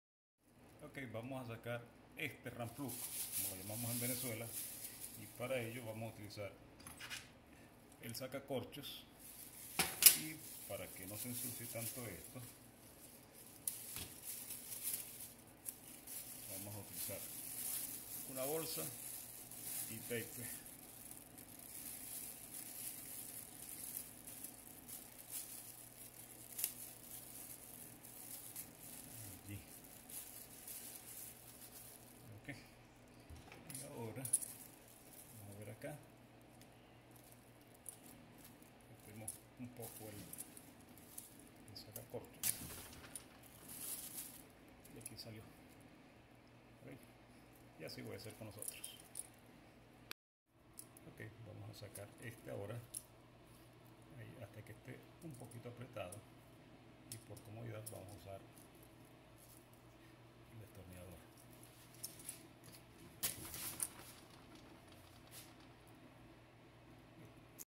Asombrosamente fácil: Recupera los Ramplugs (tarugos, espiches) usados.
Acá te presento una forma fácil y expedita para sacar ramplugs de paredes.
Una vez hecho esto, muy probablemente lo podrás usar nuevamente.